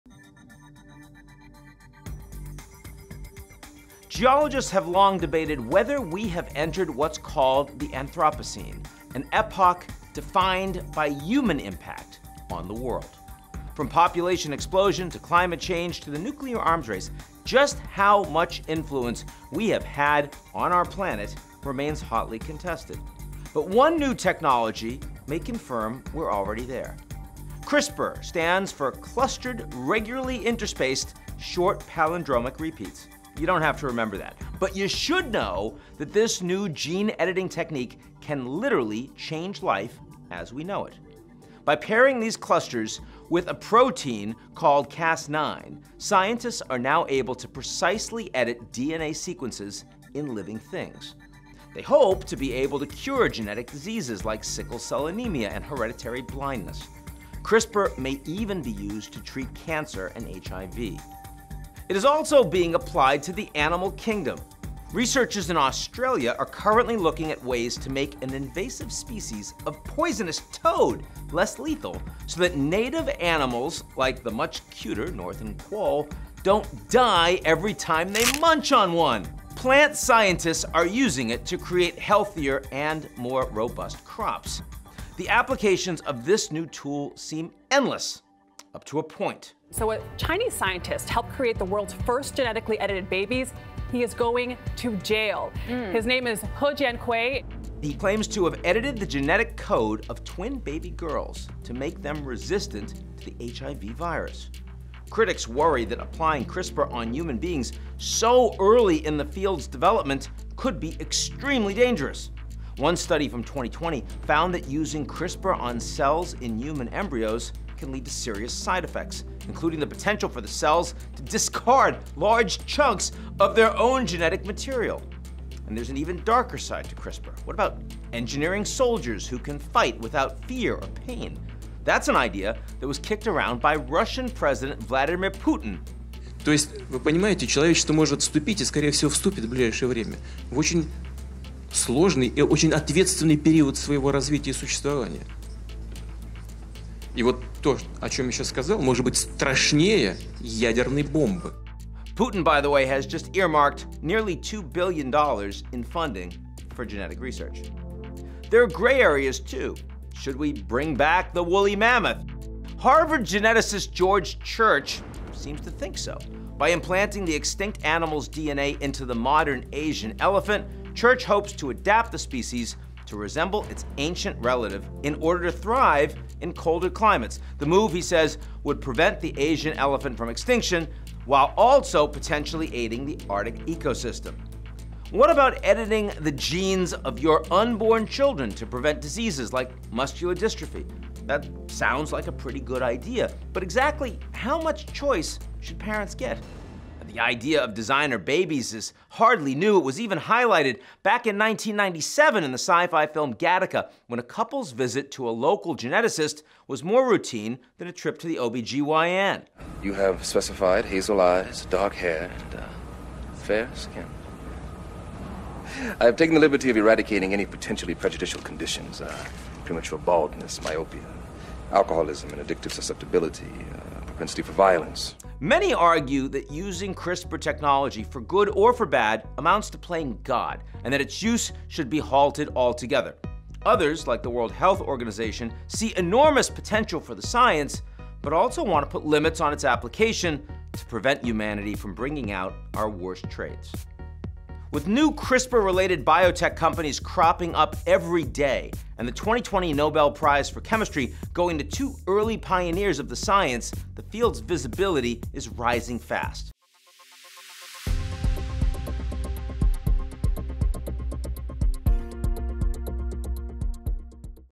Ian Bremmer Explains: CRISPR and the Gene-Editing Revolution | GZERO World
CRISPR gene editing may be used to treat diseases, but there's a darker side as well.

CRISPR stands for Clustered Regularly Interspaced Short Palindromic Repeats. You don't have to remember that, but you should know that this new gene editing technique can literally change life as we know it. Through CRISPR, scientists are now able to precisely edit DNA sequences in living things. They hope to be able to cure genetic diseases like sickle cell anemia and hereditary blindness. CRISPR may even be used to treat cancer and HIV. There’s a darker side to CRISPR. What about engineering soldiers who can fight without fear or pain? Many argue that using CRISPR technology—for good or bad—amounts to playing God and that its use should be halted altogether. Others, like the World Health Organization, see enormous potential for the science but want to put limits on its application to prevent humanity from bringing out our own worst traits. Ian Bremmer explains what we know and don’t know about the brave new world of gene editing.